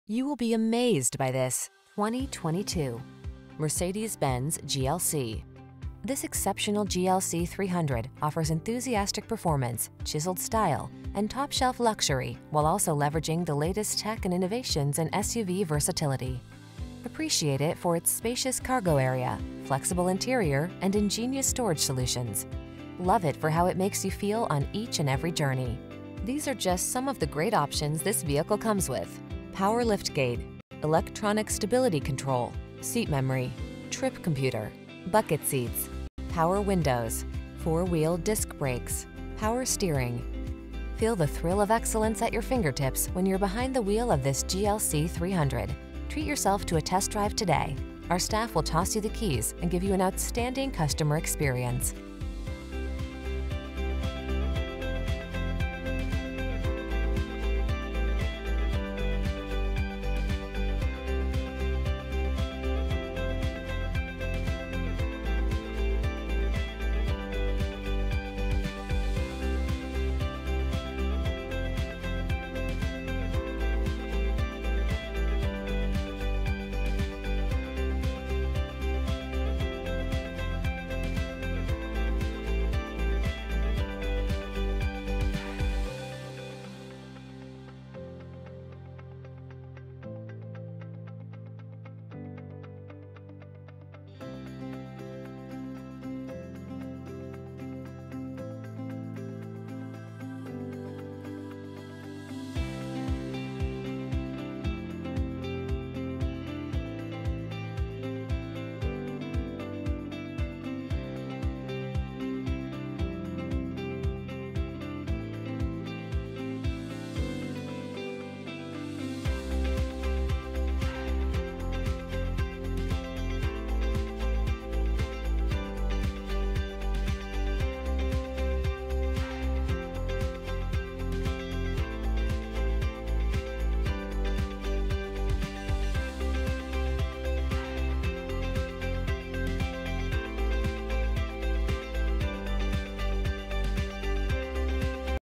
2022 Mercedes-Benz GLC Salem, NH, Newburyport. MA, Andover, MA, Haverhill, MA, Methuen, MA 21K-3652
Polar White New 2022 Mercedes-Benz GLC available in Salem, New Hampshire at Smith Motor Sales of Haverhill. Servicing the Newburyport. MA, Andover, MA, Haverhill, MA, Methuen, MA area.
2022 Mercedes-Benz GLC GLC 300 - Stock#: 21K-3652 - VIN#: W1N0G8EB9NV335118